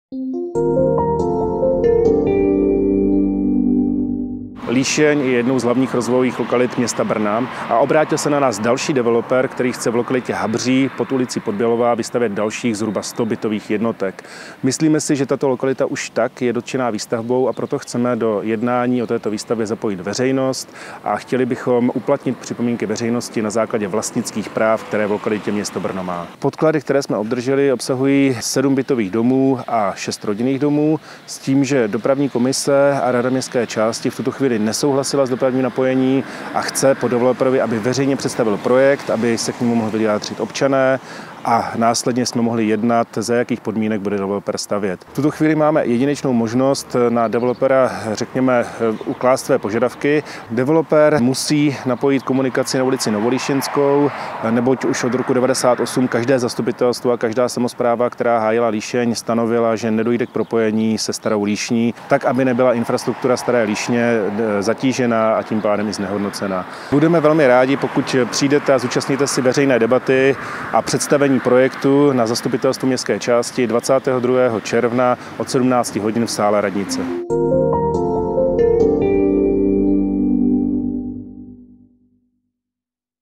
Další developer v Líšni?
V lokalitě Habří chce stavět další developer. Má tam vzniknout několik bytových a rodinných domů. Radnice proto pořádá veřejnou debatu s tímto developerem, který by měl lidem představit své plány týkající se výstavby a jejího dopravního napojení. Debata se uskuteční v rámci zasedání zastupitelstva ve čtvrtek 22. června od 17.00 ve velkém sále líšeňské radnice.